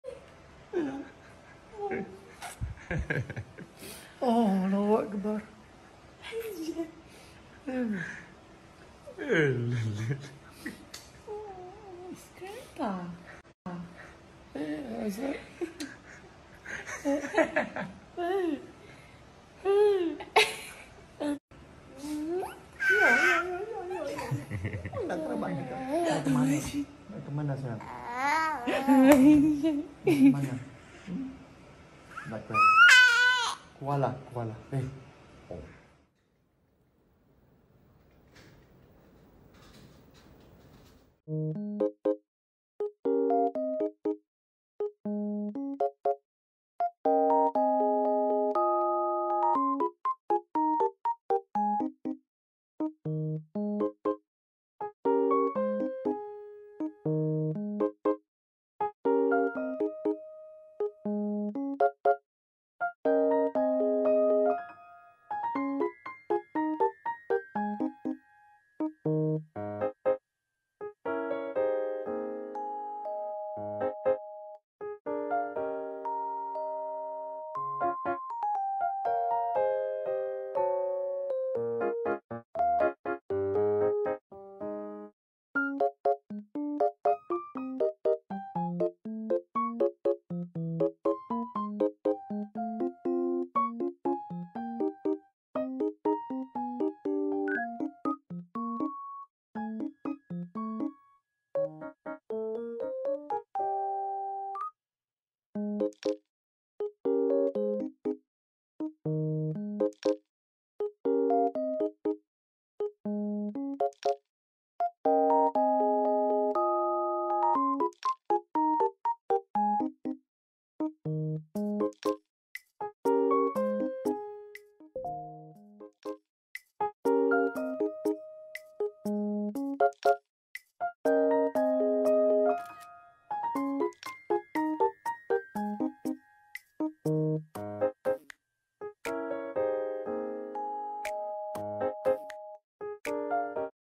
Meeting Grandpa for the first time !! Issac Raees is super cute and adorable 😍
Alhamdulillah finally “the Original grandpa” Zainol Johari Macwilson meets Grandson Issac Raees for the 1st time ever..

A grandfather is a significant person in the family history!